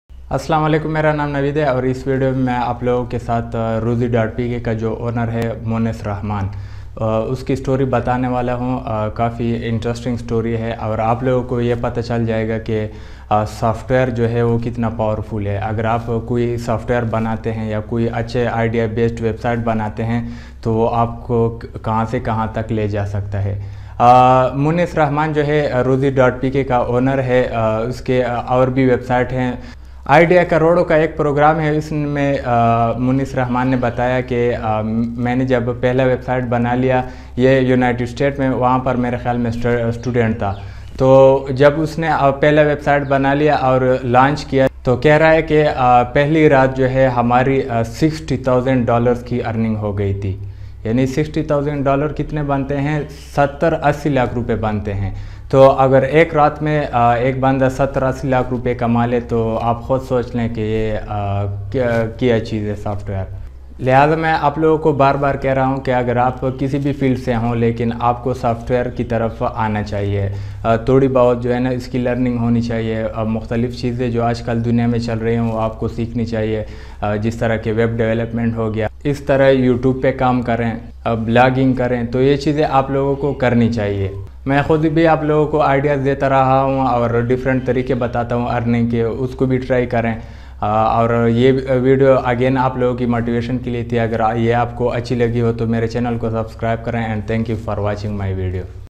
Rozee.pk Owner Motivational Story | Monis Rehman Story
Rozee.pk owner story explained in this video. This video is just for you people motivation. Monis Rehman is the owner of rozee.pk.